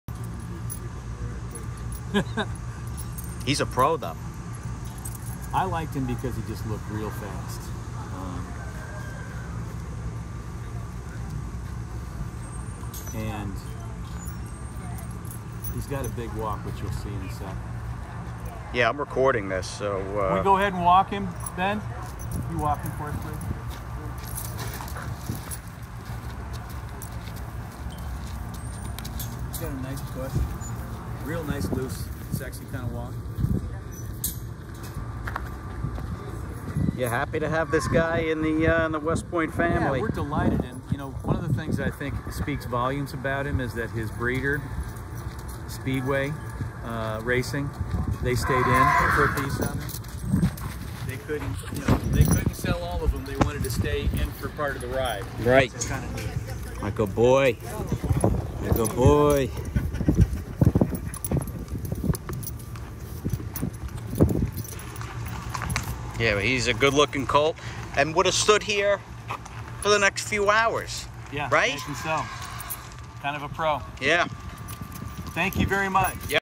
Our new yearling: Medaglia d’Oro - Leigh Court, by Grand Slam (Keeneland September)
Consignor Arthur Hancock gave this colt his blessing and stamp of approval.